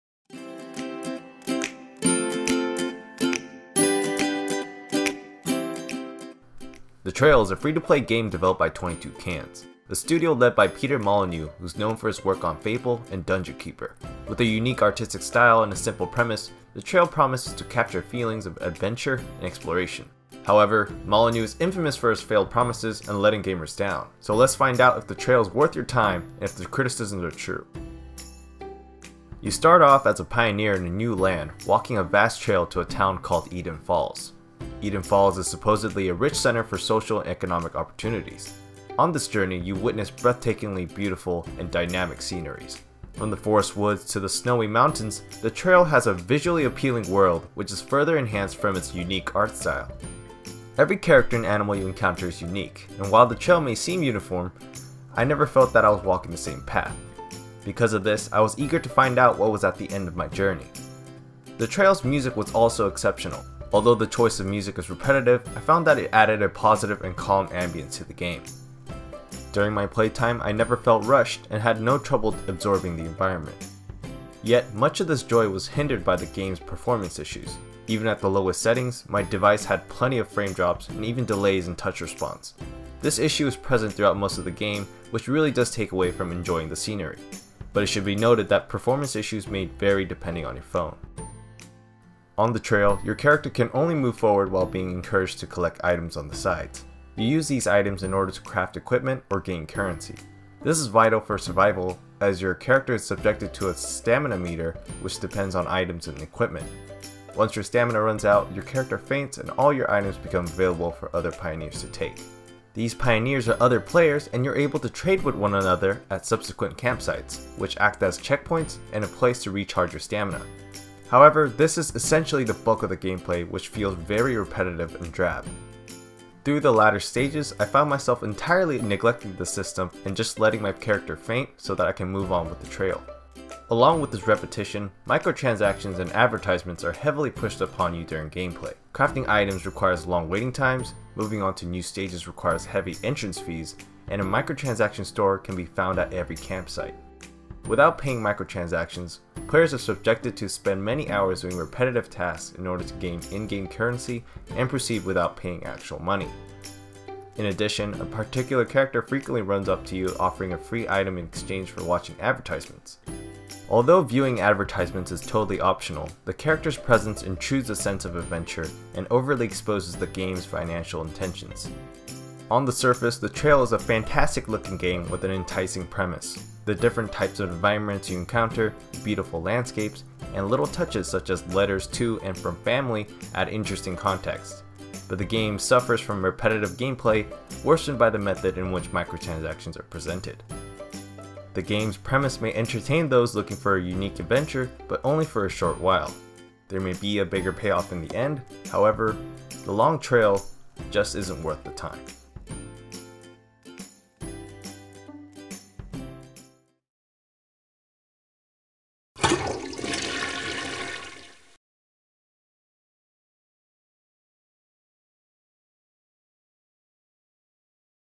The Trail - Review Dump
The Trail is a free-to-play game created by Peter Molyneux and his studio, 22Cans, that attempts to capture the sense of adventure and exploration. There are great elements within the game, however, many of its core mechanics fall short.

All review scores are out of 10.

Credits:
Written by - Ho Young Won
Narrated by - Ho Young Won